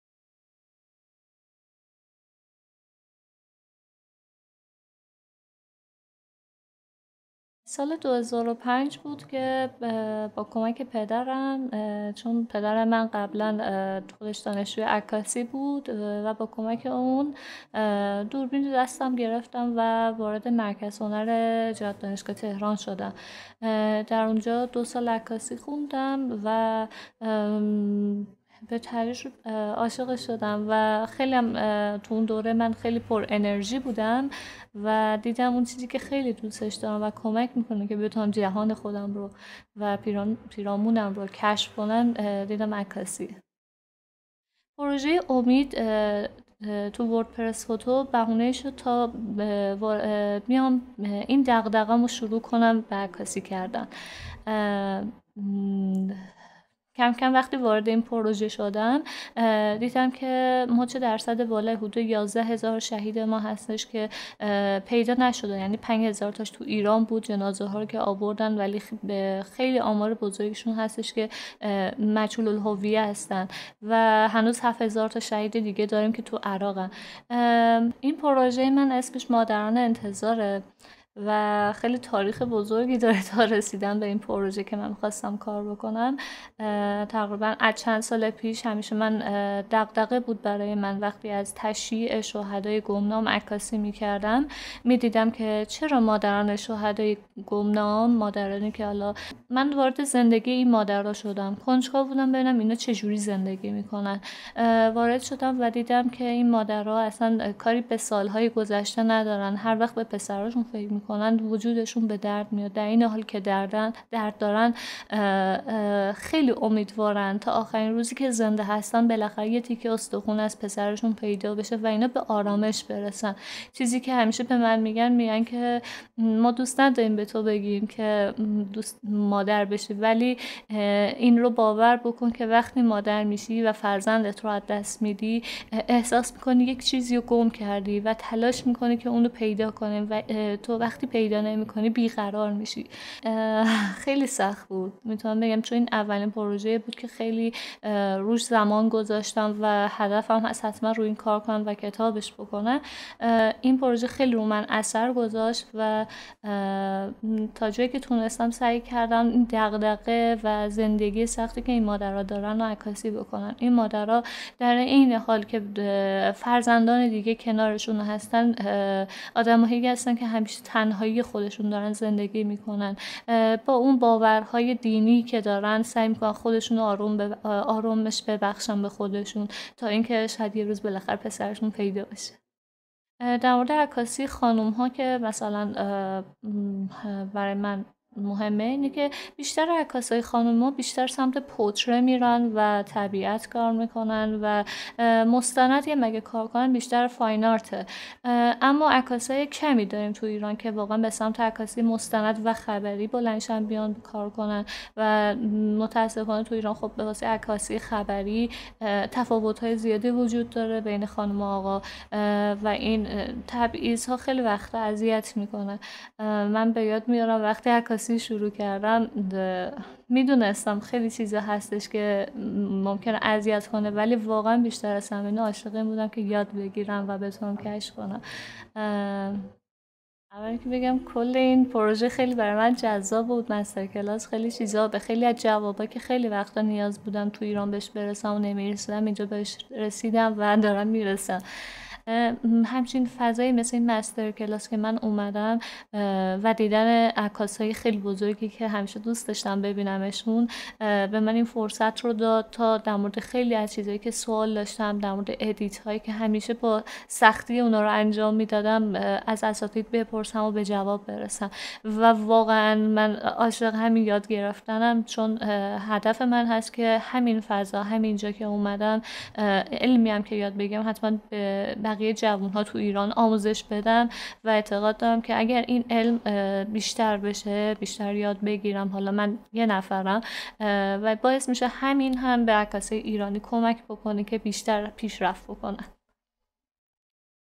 Fatemeh Behboudi (2013 Joop Swart Masterclass)
2013 Joop Swart Masterclass participant Fatemeh Behboudi discusses her photo story, Mothers of Patience.